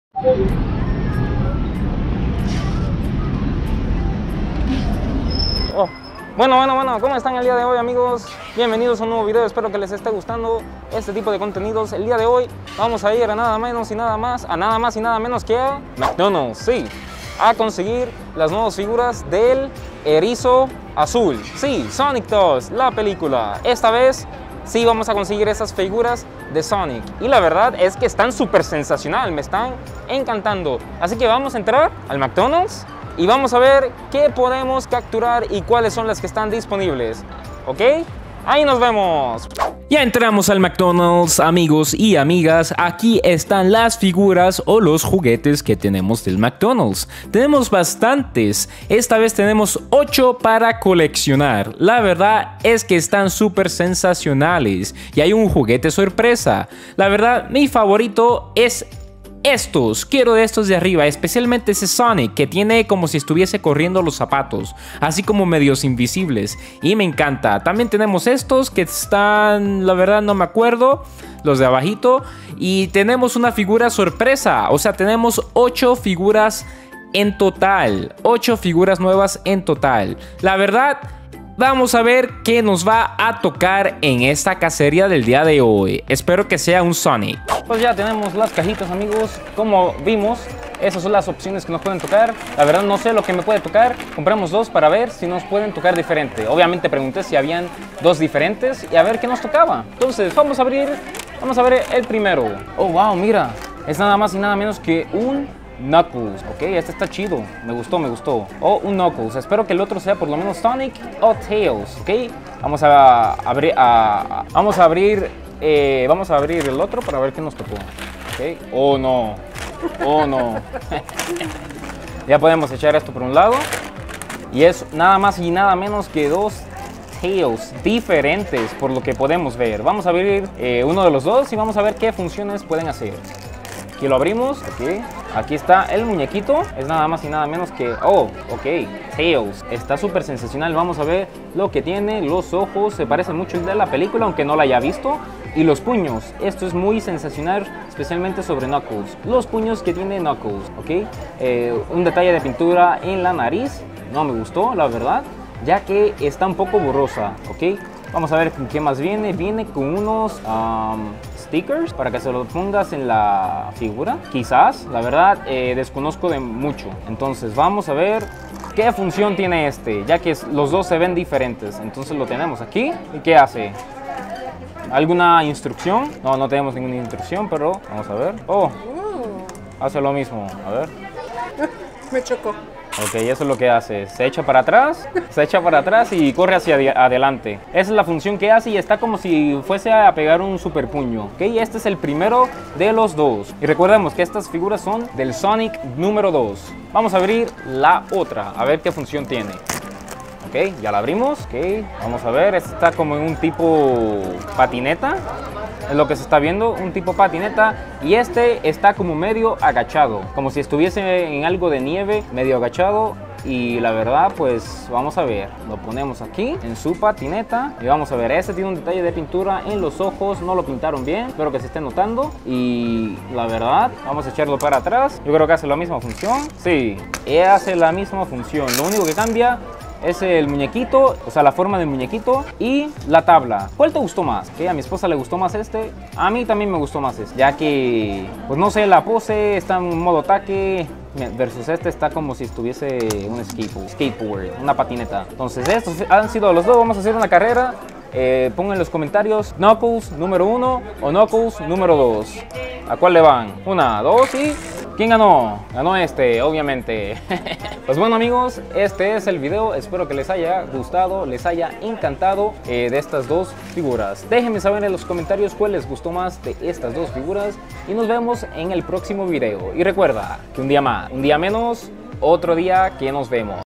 Sonic 2 en la CAJITA FELIZ de McDonald's! | Cacería
Ya están disponibles las figuras de Sonic 2 en McDonald's! Estos juguetes de Sonic 2 están super sensacionales, en especial la función que tienen! Los juguetes son de la nueva película de Sonic la 2. Y nos toco 2 figuras en un McDonald's! Estas 2 figuras son de Knuckles! Y tenemos a Tails y a Sonic.